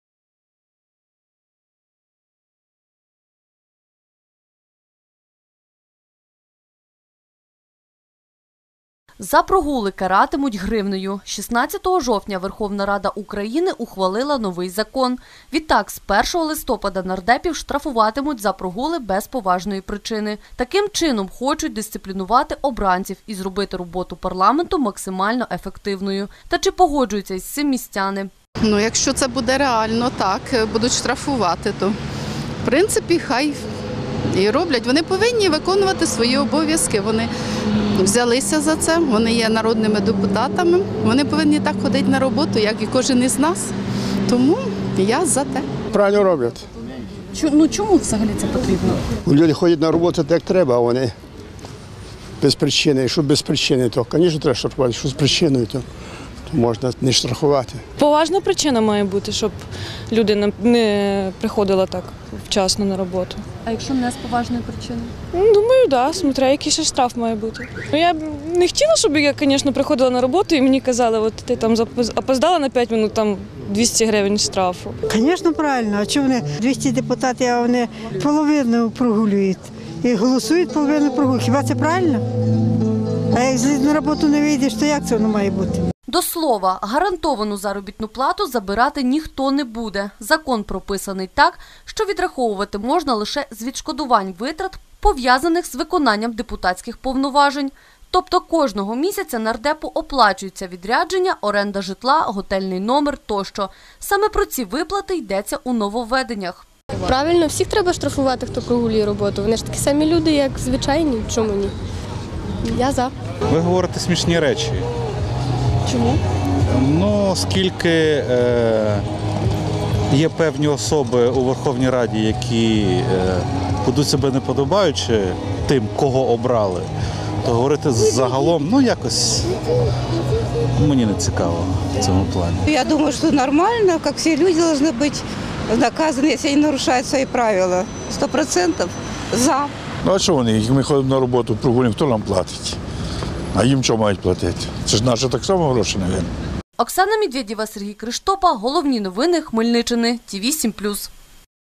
З 1 листопада нардепів штрафуватимуть
Закон за законом. Місяць тому народні депутати України проголосували за зняття недоторканості. Його на практиці ще неможливо застосувати. Тож до обранців взялися з іншого боку. З 1 листопада за прогулювання роботи нардеп може залишитися без премій та доплат.